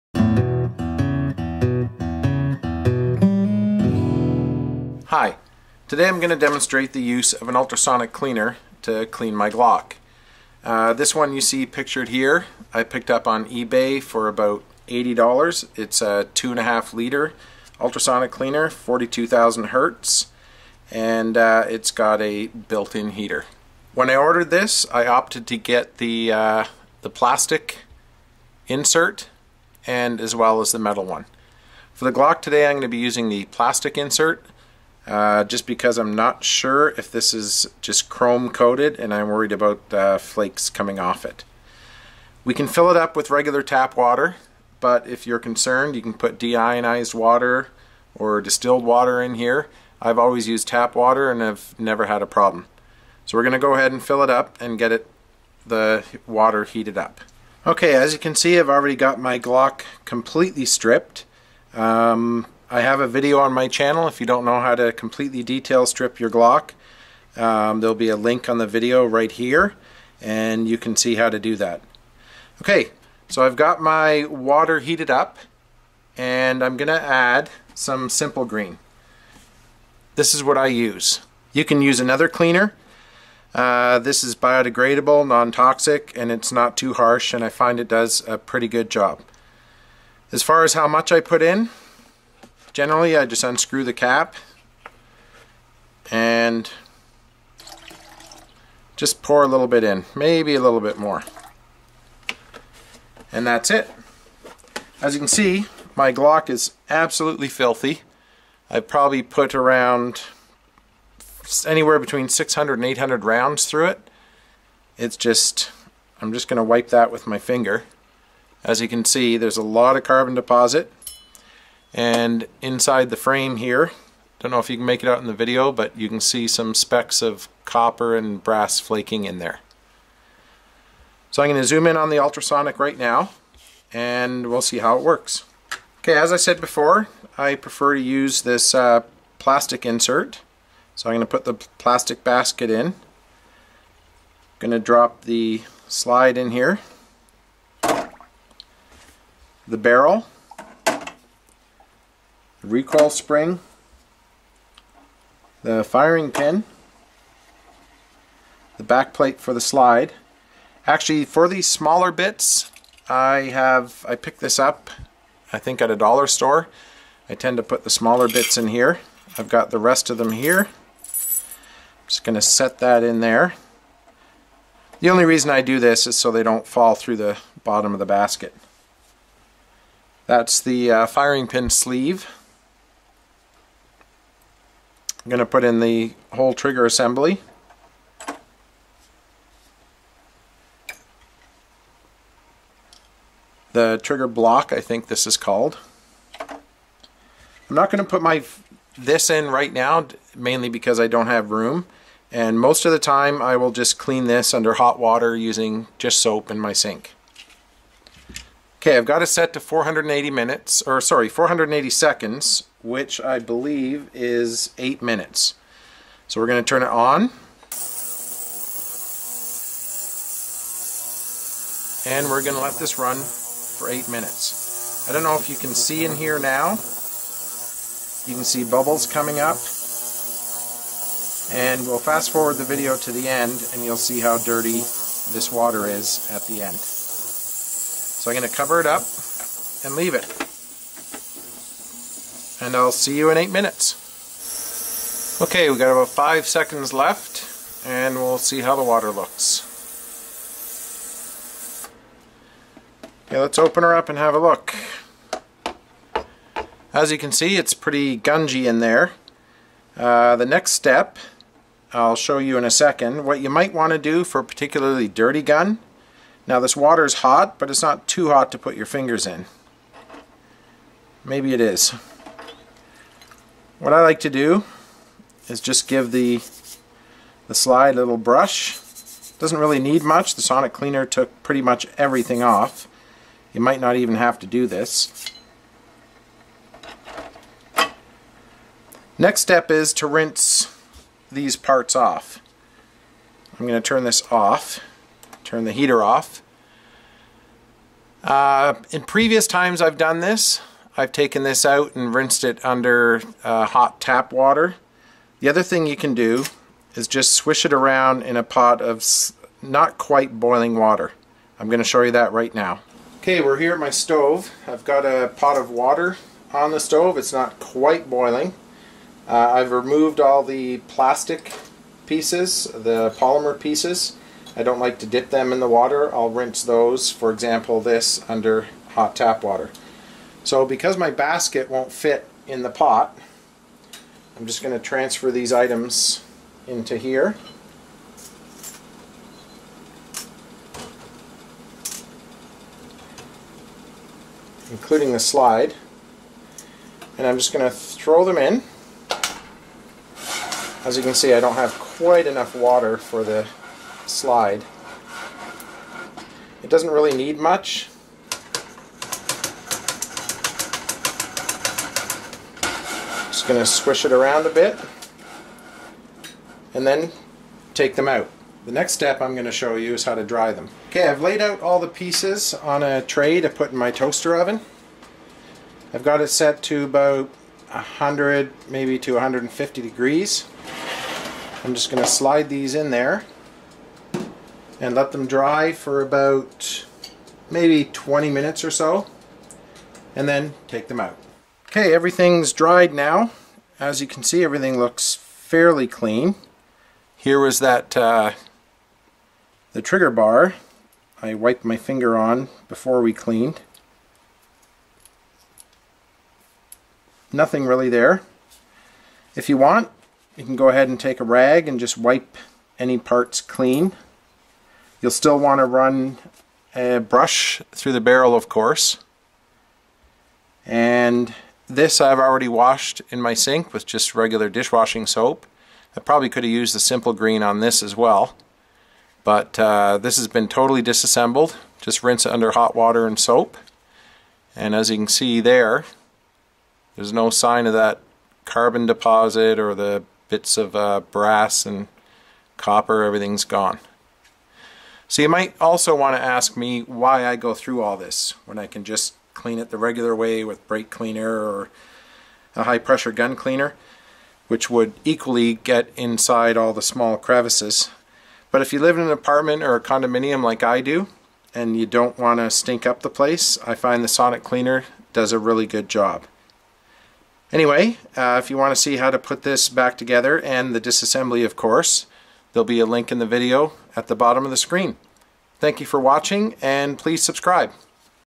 Ultrasonic Cleaner - Glock 17
Demonstration of an Ultrasonic Cleaner being used for cleaning my Glock 17 Gen4.